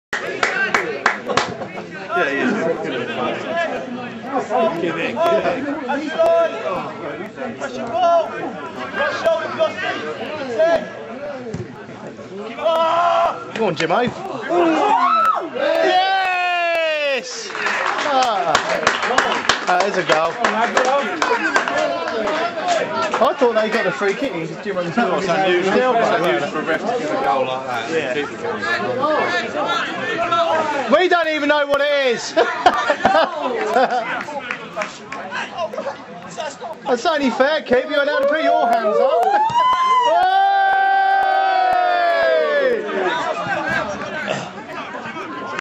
Soham Town Rangers vs Harwich + Parkston 4
Ridgeons Premier League 20th October 2007.

The last goal from Paul Jones in the 4-0 win for the Greens.